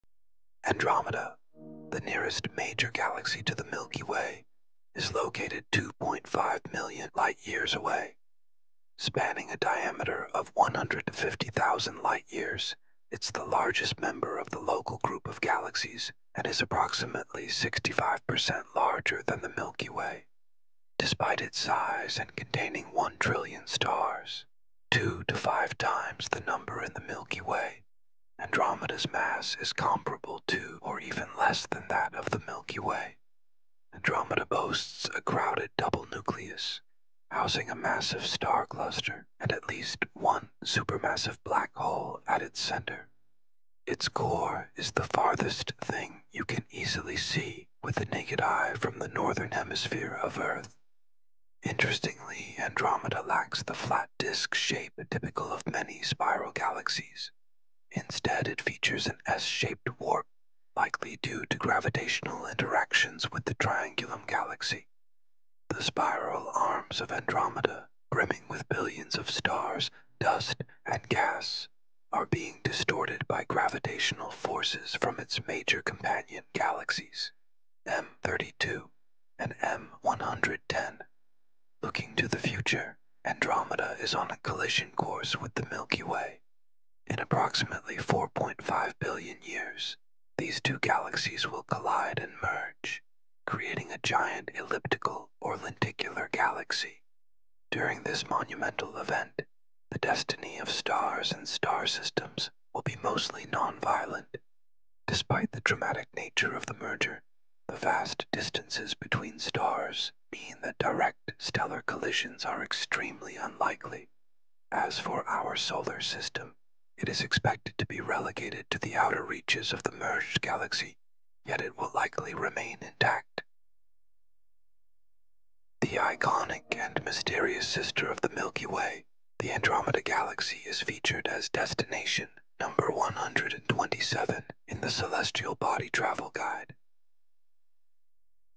🌀✨ Andromeda🌌👯‍♀️​
The Milky Way Sister Galaxy 👭​

➡️ The nearest major galaxy to the Milky Way 2.5 million ly from Earth 🌍📏
➡️ The largest member of the Local Group (220,000 ly in diameter) 🌌📏👑
➡️ Barred spiral galaxy twice the number of the stars of the Milky Way 🌟🌟💫💫
➡️ S-shaped warp (rather than a flat disk) possibly by interaction with Triangulum Galaxy 🌀🔄🤝
➡️ In 4.5 billion years it will collide with the Milky Way merging in a giant elliptical or a lenticular galaxy 💥🌌💏➡️🪐👀

credits: NASA/Wikipedia/Universe Guidebook 🔭 POSTERS 🌌 and BOOKS 📖 available in their bio link 👉 @universeguidebook